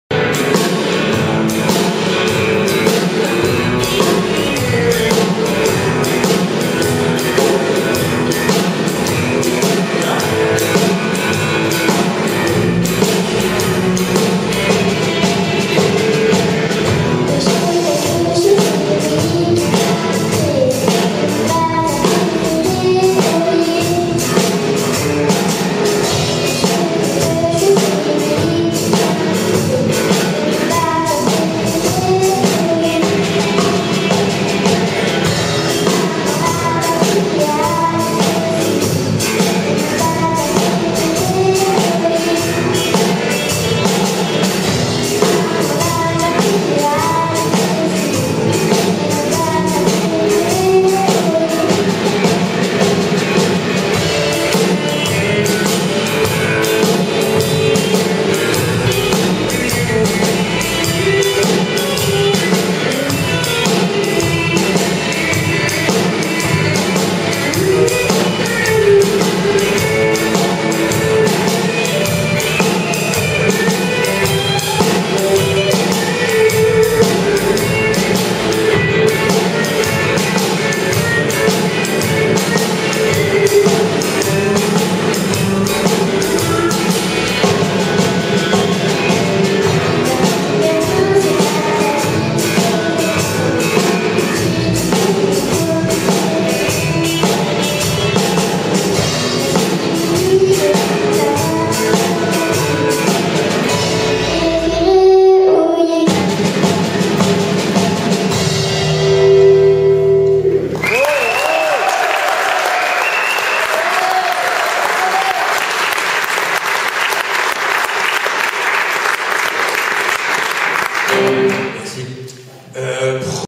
Lilix & Didi bluesy rock band au festival Blues à Senart - St Pierre du Perray
L'atelier est de sortie dans le cadre du festival BLues à Senart.
Pour l'occasion, on a fait un peu de blues ;o)